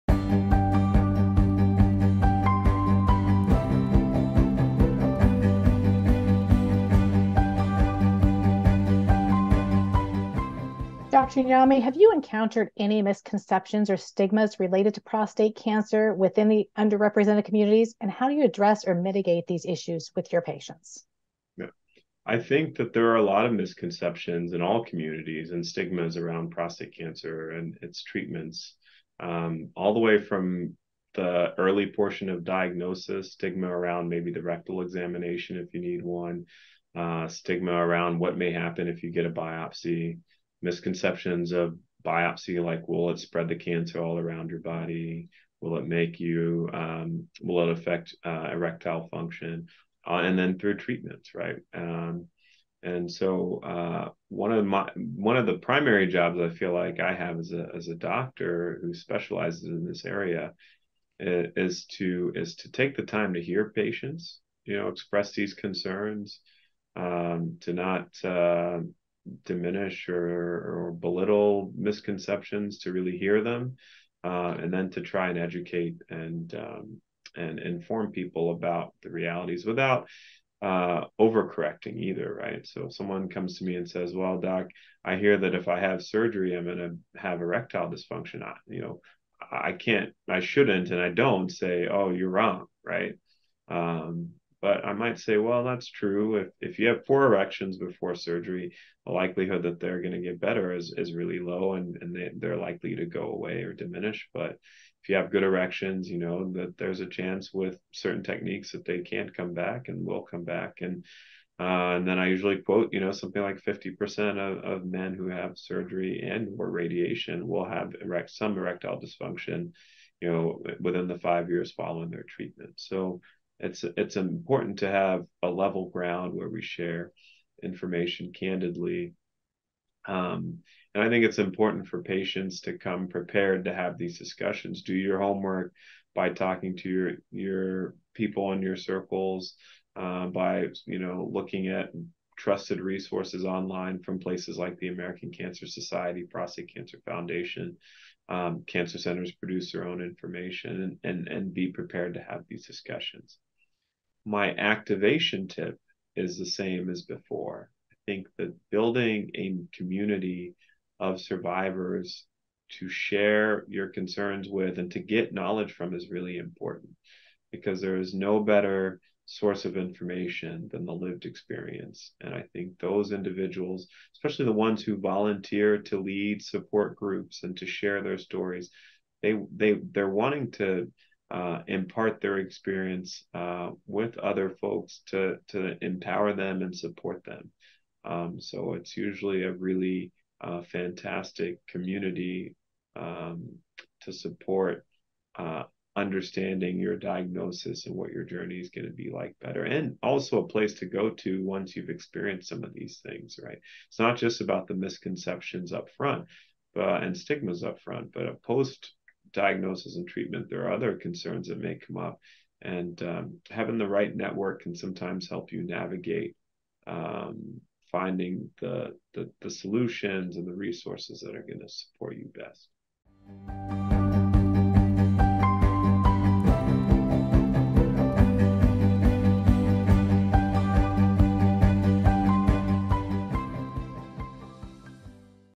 How Can Prostate Cancer Stigmas and Misconceptions Be Addressed?
How can prostate cancer stigmas and misconceptions be addressed? Expert Dr. Yaw Nyame with the University of Washington discusses common concerns and misconceptions, how doctor-patient communication can help, and support resources to aid patients and care partners.